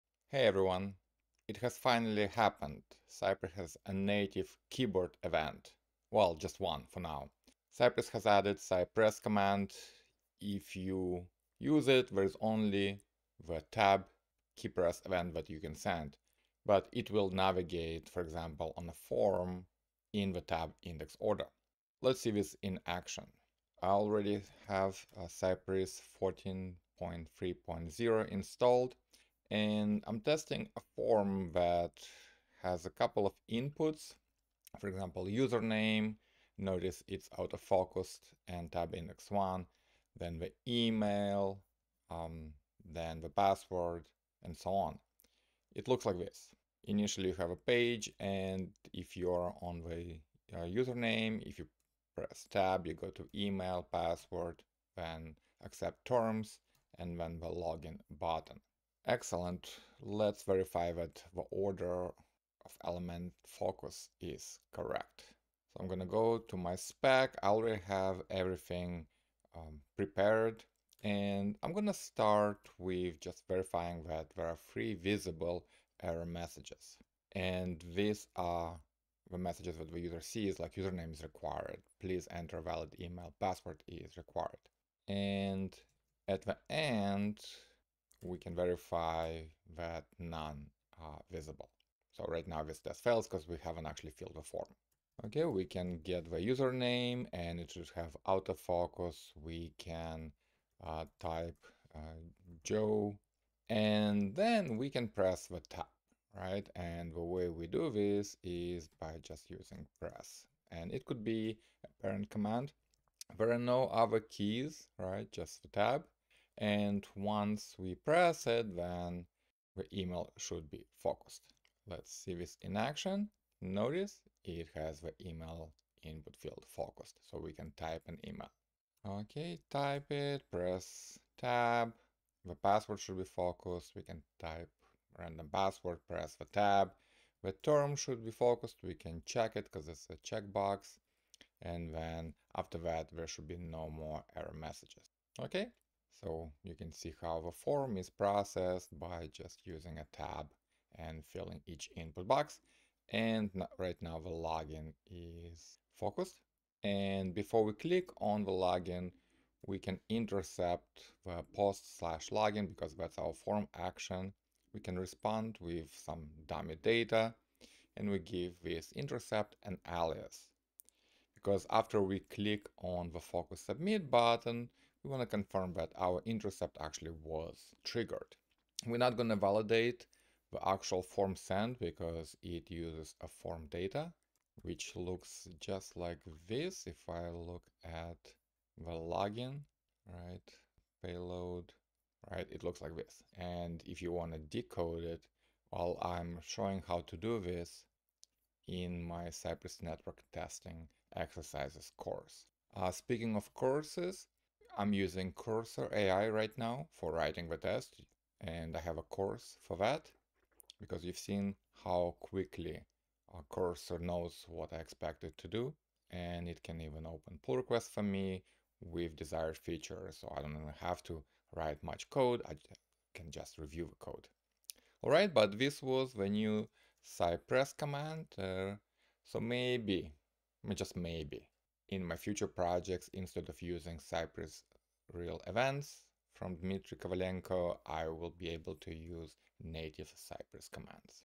Demo Of The New cy.press Command In Cypress v14.3.0
Cypress v14.3.0 includes the new cy.press command that send the native "Tab" key press to the window and lets you navigate the DOM elements in the "tabindex" order. In this video, I write a Cypress test that fills the login form using "cy.press" and "should be focused" assertions.